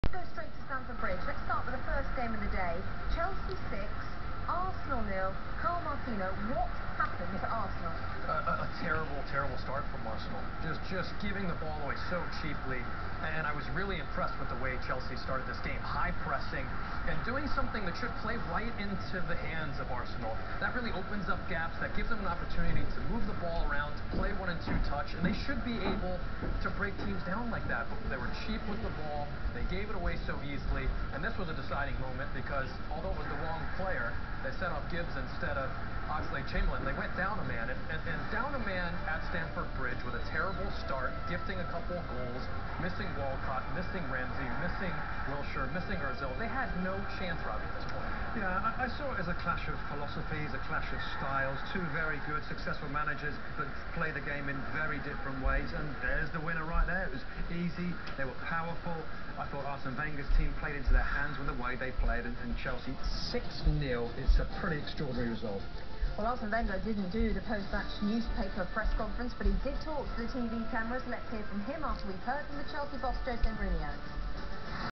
Chelsea FC Vs. Arsenal FC Highlights: March 22, 2014
Chelsea FC beats Arsenal FC 6-0 in a blowout match.